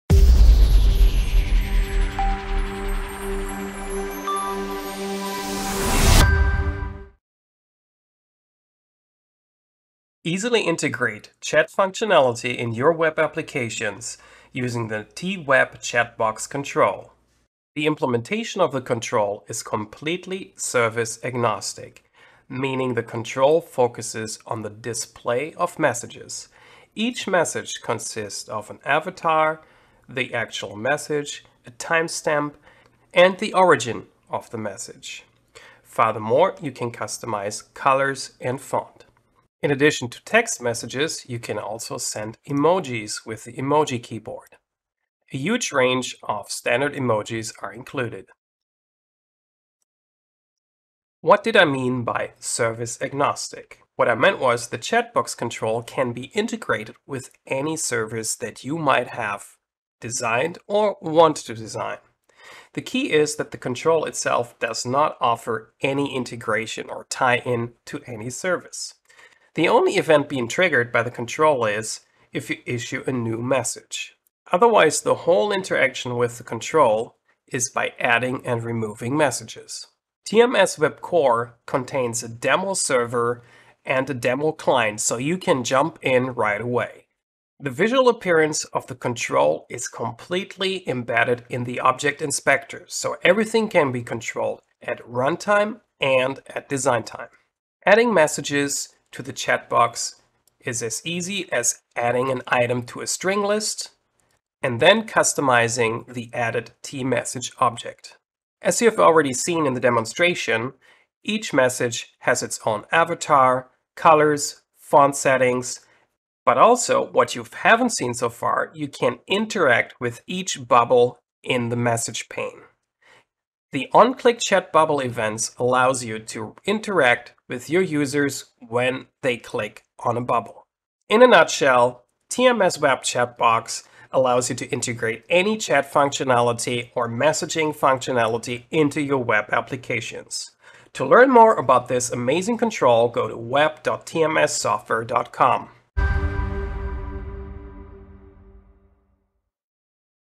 TMS WEB Core: TWebChatbox control
Discover the power of seamless chat integration with the TWebChatBox Control! Easily incorporate chat functionality into your web applications without the hassle of service tie-ins. With customizable features including avatars, fonts, and colors, and the ability to send emojis, this control offers a dynamic user experience. Explore how each message bubble can be interactive, engaging your users like never before.

ABOUT:
TMS software, established in 1995, is a software development company specialized in: